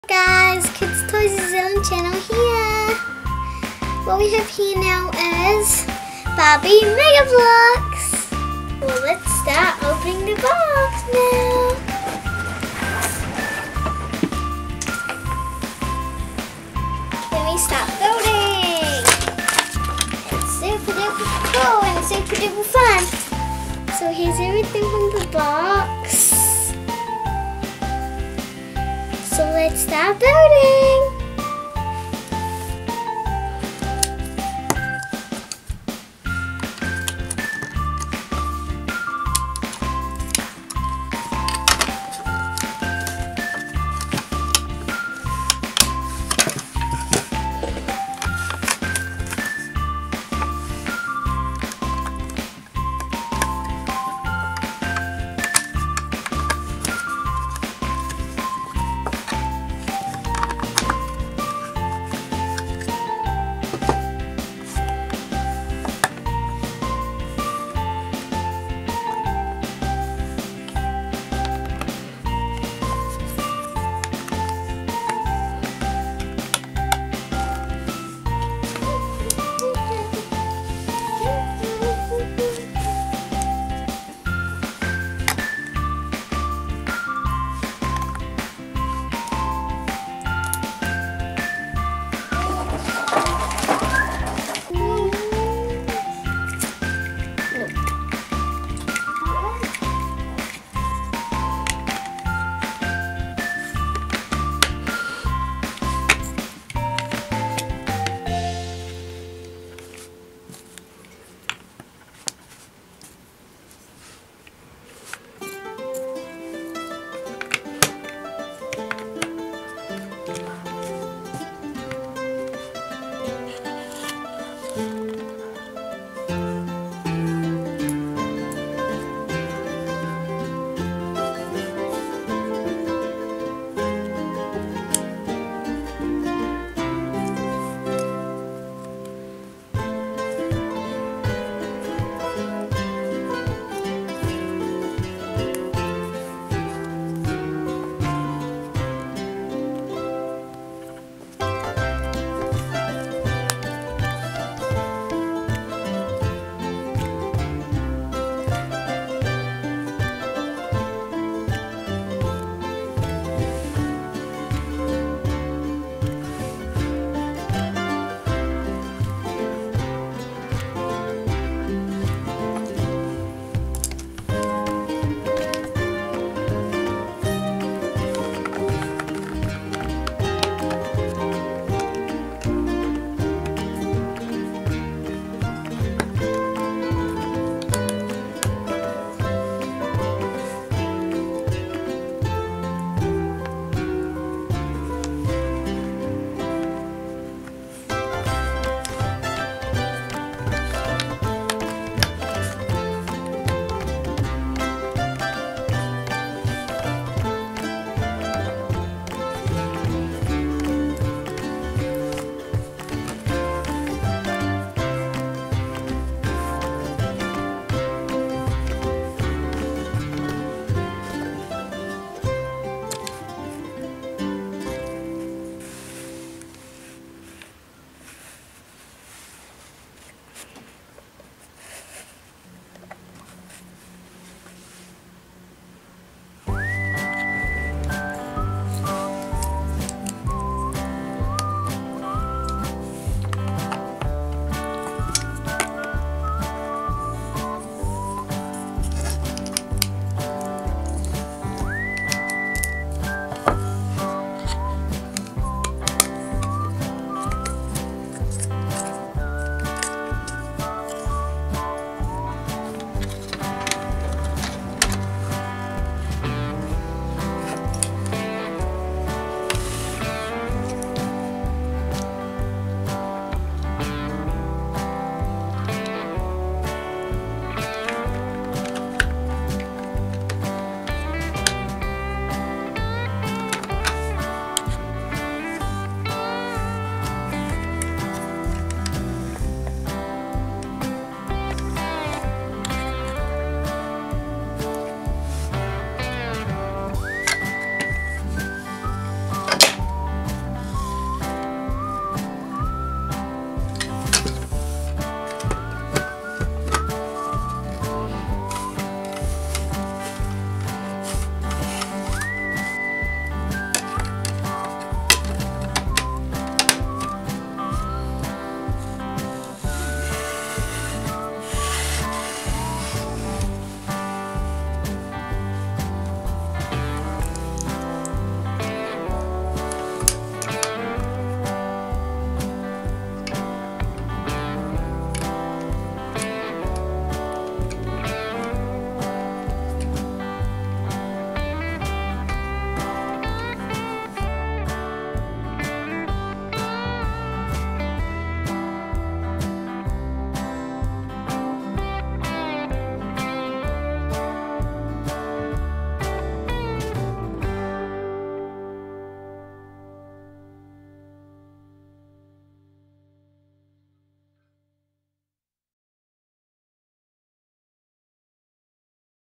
Barbie Build n' Style Beach House Mega Bloks Barbie and Nikki l Kids Balloons and Toys
Toy Channel Kids Balloons and Toys NZ presents Barbie Build n' Style Beach House Mega Bloks.

It's time for beach fun Barbie to take a Super Fun vacation at the Mega Bloks Barbie Build and Style Beach House! Let the beach party begin! Turn up the music and beach fun Barbie and her friend Nikki mini fashion figures really spin on the whirl and twirl dance floor! There's even a hot tub that can really hold water! Show your summer style and have fun at this fabulous Barbie beach house!

Mega bloks beach house has two rooms and a patio to build and style! It includes beach fun Barbie and her friend Nikki mini figures with changeable outfits!

Combine with all of the Barbie Mega Bloks Playset to build a fabulous world of fashion, fun and friends!